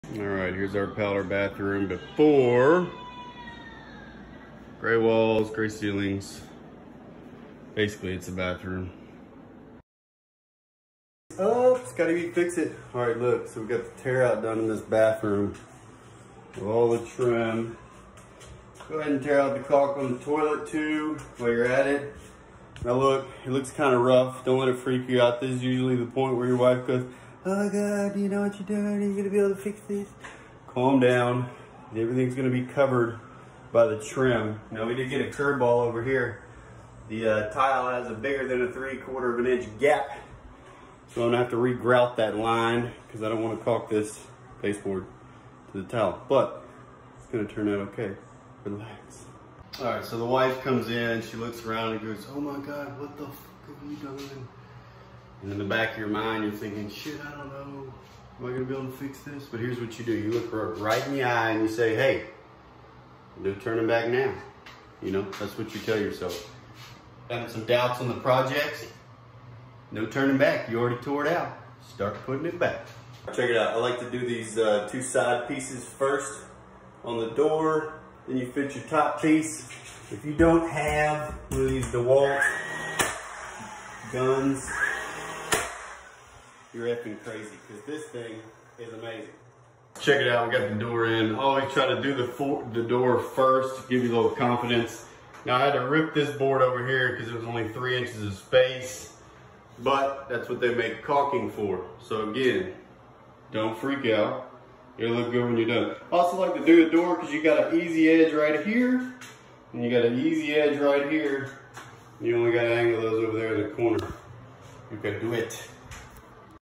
DIY Powder Bath Makeover
Powder bath Project. Quarantine 2020.  New trim and paint makes a world of difference.
Tear out and trim start. Day 1
Wait til you see it.